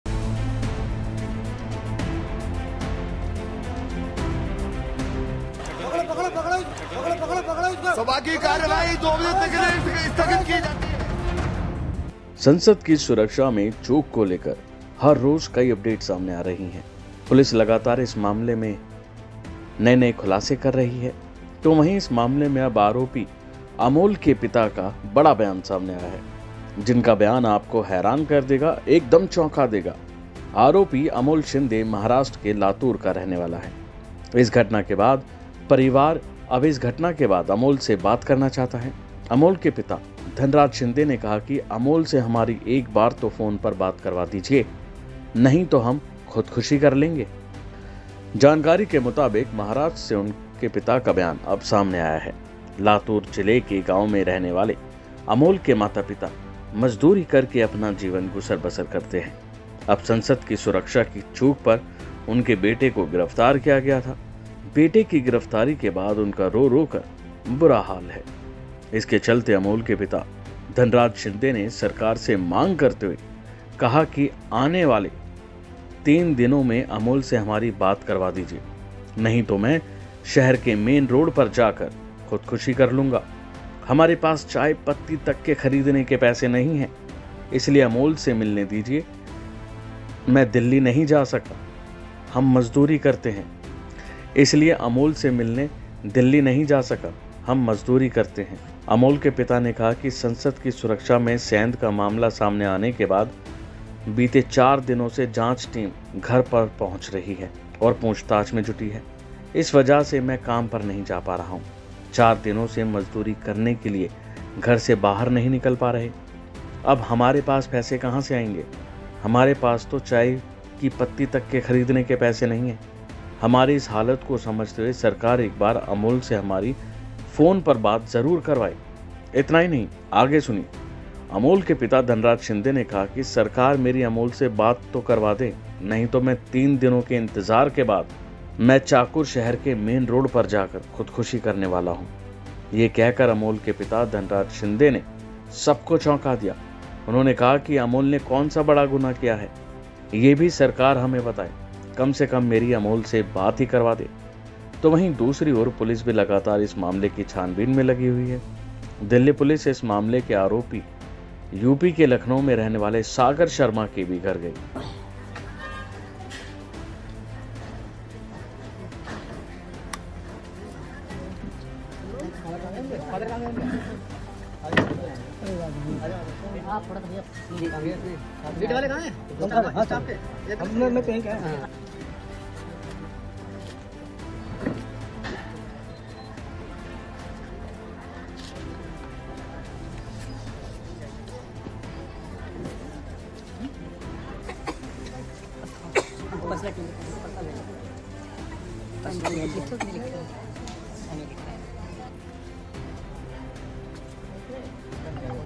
Parliament Security Breach: आरोपी Amol के पिता Dhanraj Shinde के बयान से सब हैरान | वनइंडिया हिंदी
Parliament Security Breach: संसद की सुरक्षा में हुई चूक के मामले में (Police on Amol's Father) पुलिस दिन रात छानबीन कर रही है। आरोपियों के घर से पूछताछ करने में जुटी है। मामले में अब आरोपी अमोल के पिता का बयान सामने आया है। जिनका बयान एकदम चौंकाने वाला है। आरोपी अमोल शिंदे (Amol Shinde) महाराष्ट्र (Maharashtra) के लातूर (Latur) का रहने वाला है. इस घटना के बाद वे अमोल से बात करना चाहते हैं. अमोल के पिता धनराज शिंदे (Dhanraj Shinde) ने कहा है कि अमोल से हमारी एक बार तो फोन पर बात करवा दी जाए, नहीं तो हम खुदकुशी कर लेंगे।